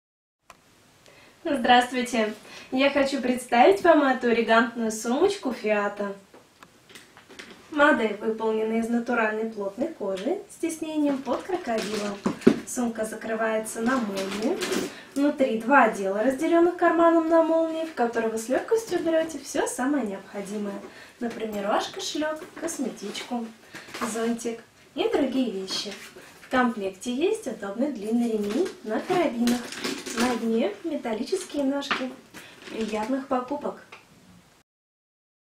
Сумка Fiato 2986 croc brown
Эта элегантная сумка из натуральной кожи с тиснением под кожу крокодила найдет поклонниц среди женщин любого возраста. Прекрасно подойдет как к деловому костюму так и к гламурной блузке. Стильная и очень удобная модель закрывается на молнию, носится на сгибе руки или на плече. На дне имеются металлические ножки. Хорошо держит форму.
Внутри: два отдела, разделенных карманом на молнии. На задней стенке карман для документов на молнии, на передней два кармана для мелочей.
Снаружи: карман на молнии на задней части сумки.